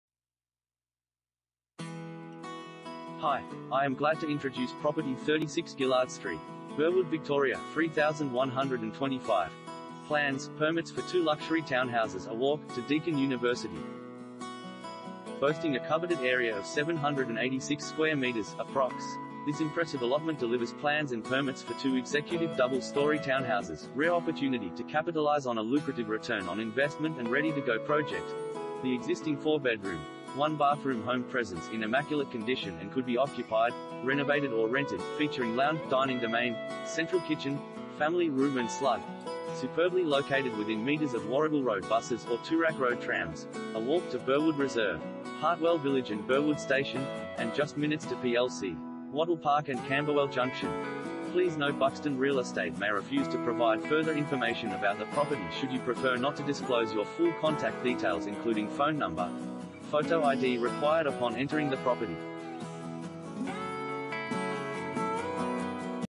36 Gillard Street, Burwood VIC 3125 | AuPropertyReport.Com
36 Gillard Street, Burwood VIC 3125
Property Type: house
Bedrooms: 4
Bathrooms: 4
Car Space: 2
Plans/Permits For 2 Luxury Townhouses A Walk To Deakin University